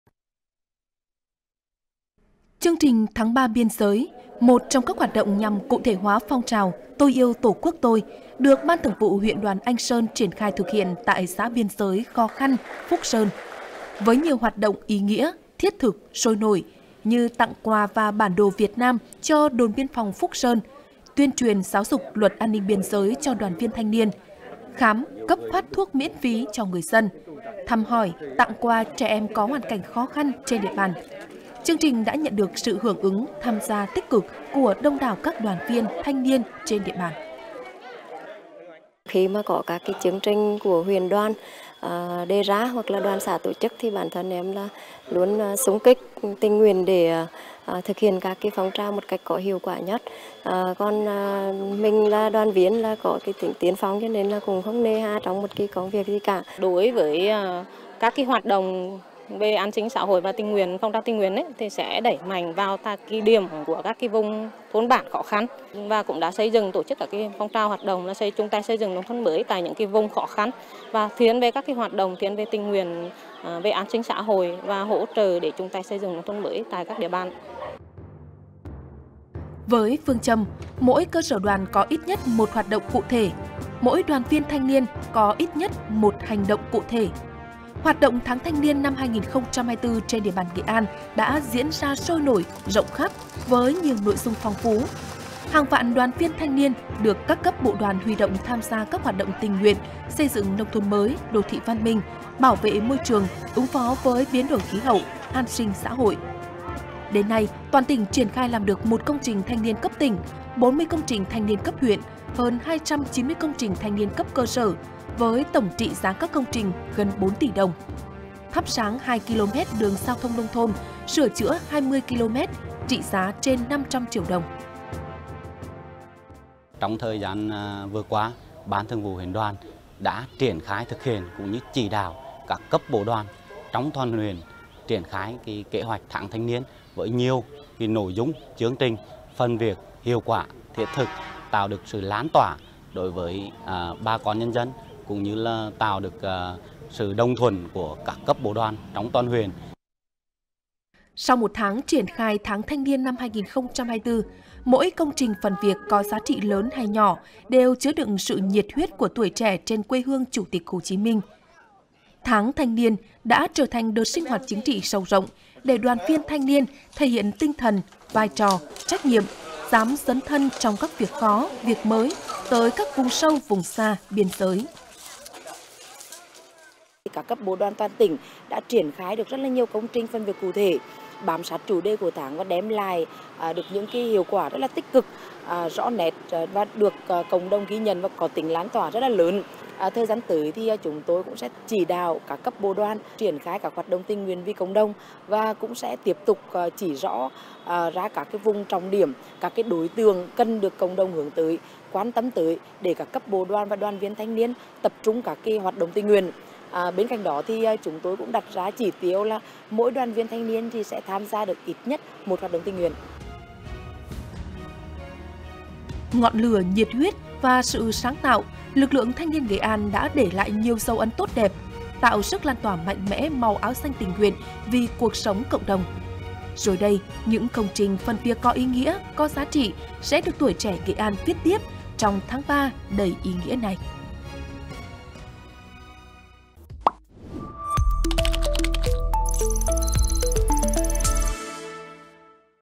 Tuổi trẻ Nghệ An xung kích vì cuộc sống cộng đồng
Hôm nay 26/3 ngày thành lập Đoàn TNCSHCM và tháng 3 cũng là tháng thanh niên. Tháng thanh niên năm nay, Trung ương Đoàn lựa chọn chủ đề là “Thanh niên xung kích tình nguyện vì cuộc sống cộng đồng”. Chỉ sau 1 tháng phát động, tuổi trẻ Nghệ An đã và đang phát huy hiệu quả tinh thần xung kích, tình nguyện đảm nhận, tham gia nhiều công trình, phần việc có ý nghĩa... Qua đó, góp phần lan tỏa mạnh mẽ nhiệt huyết cống hiến trong đông đảo đoàn viên, thanh niên.